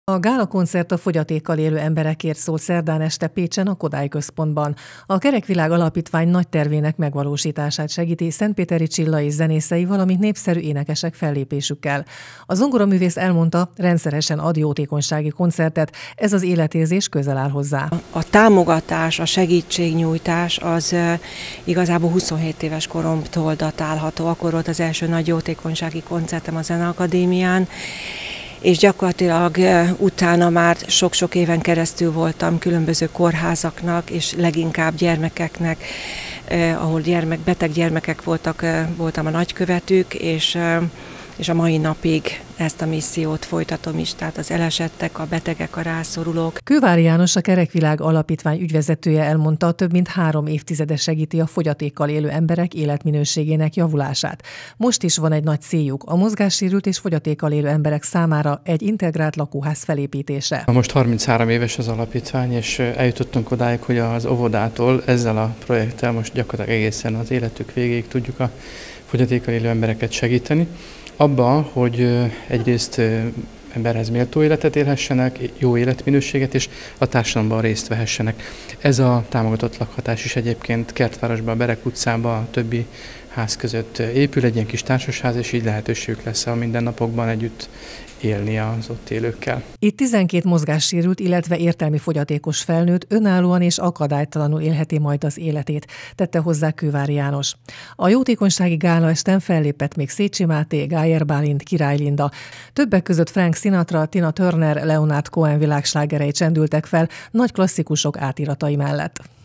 Jótékonysági gálakoncert a Kerek Világ Alapítvány javára
Nagyszabású jótékonysági gálakoncertet tartatott Szentpéteri Csilla zongoraművész és zenésztársai Pécsen a Kodály Központban május utolsó napján. A bevételt a Kerek Világ Alapítvány javára fordítják. A szervezet célja, mozgássérült és fogyatékkal élő emberek számára egy integrált lakóház felépítése.